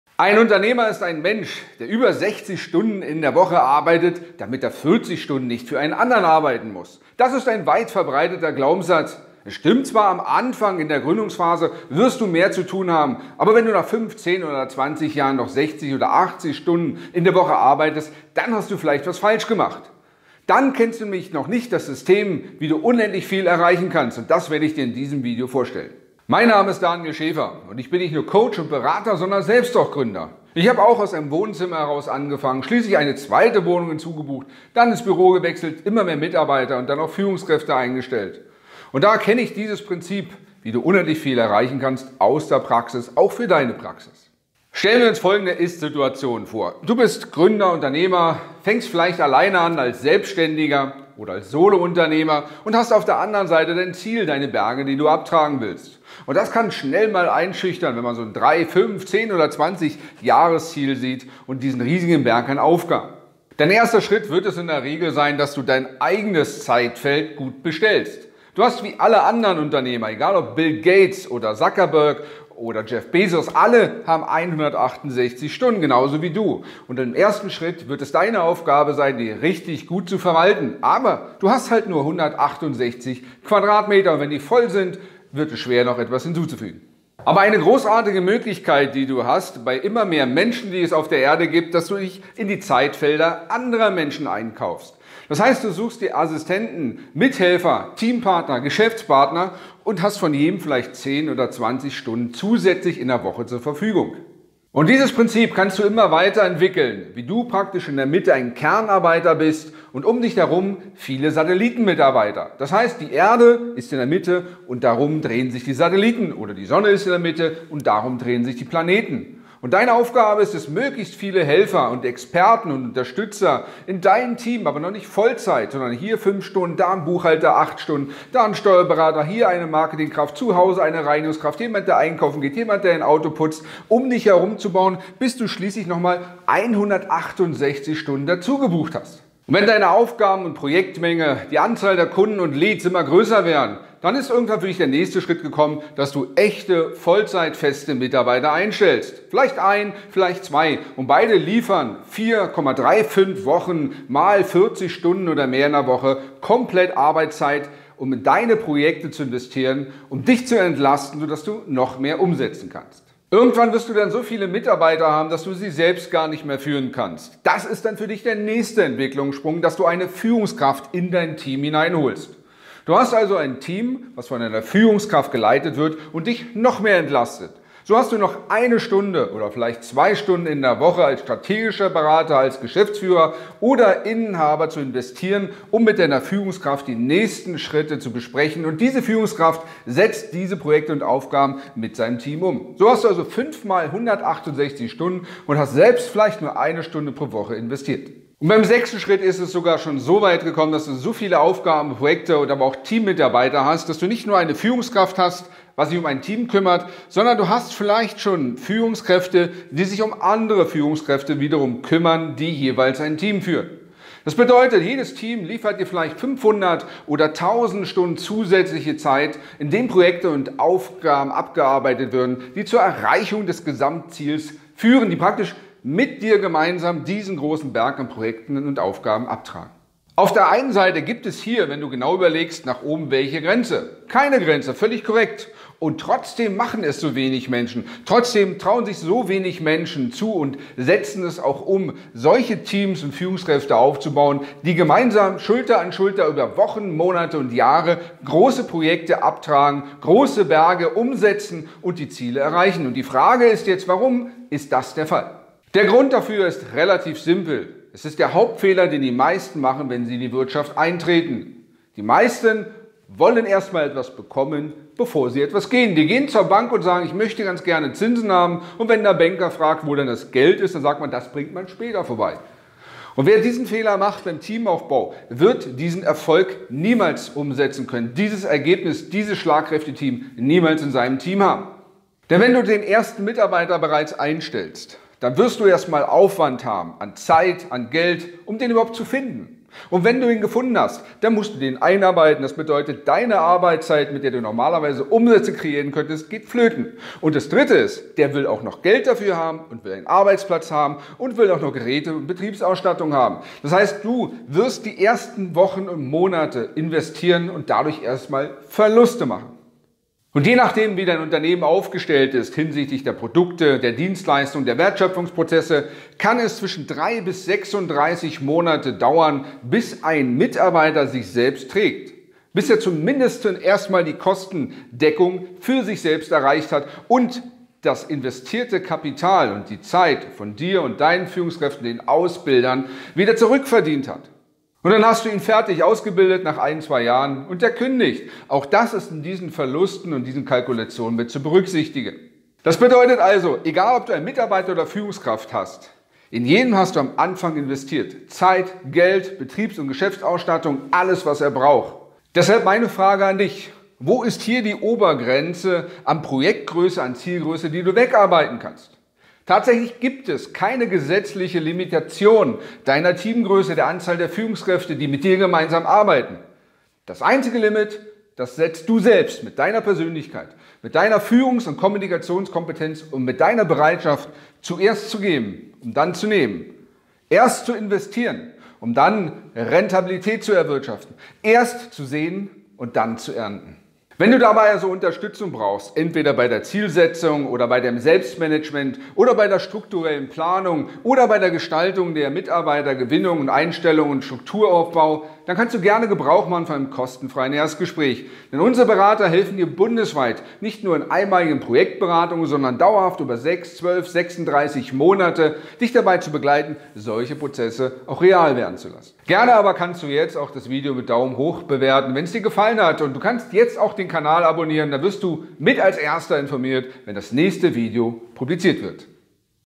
So kannst du unendlich viel erreichen!
Glaubst du, dass du alles erreichen kannst, was du dir vorstellen kannst? Oder halten dich fehlende Zeit oder fehlendes Können noch davon ab alles zu erreichen? Dann hat Daniel Schäfer in diesem Clip die Lösung für dich!